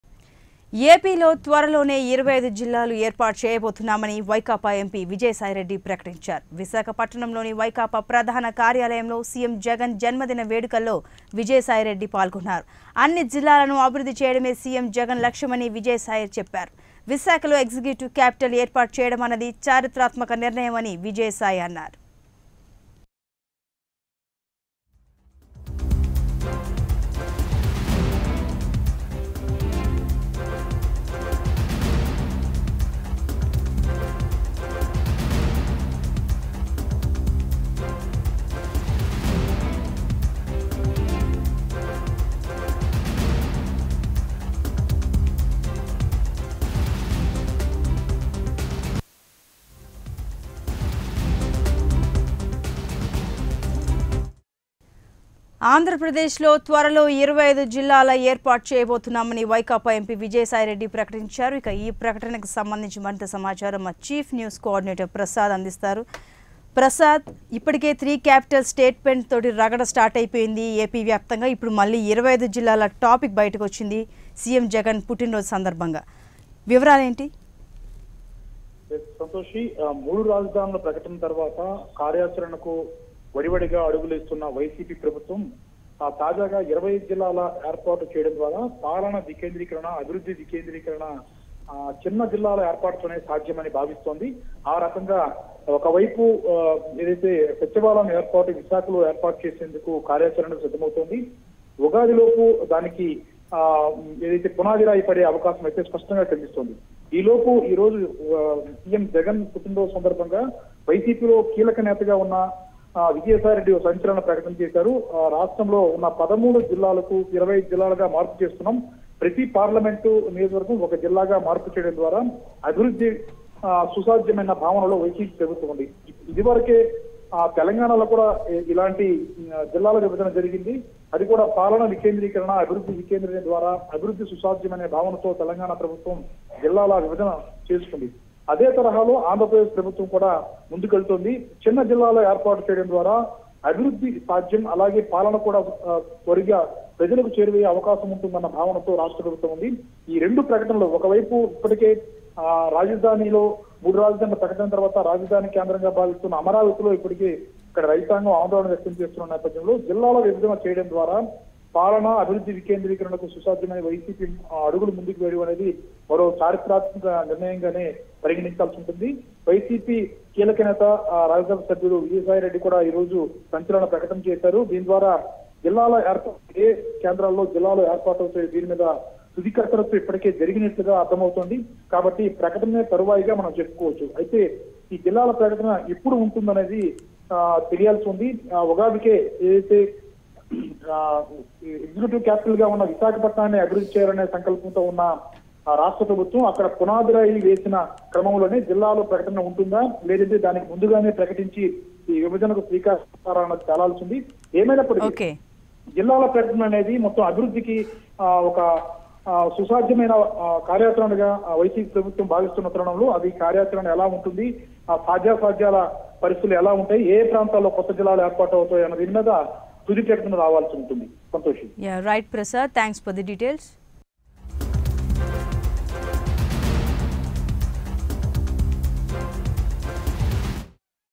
ఏపీకి త్వరలో 25 కొత్త జిల్లాలు | AP Districts To Increase From 13 to 25 | Prime9 News
This video is about Vijaysai Reddy has participated in YS Jagan birthday. YS Jagan strongly said he want to develop 25 new districts soon.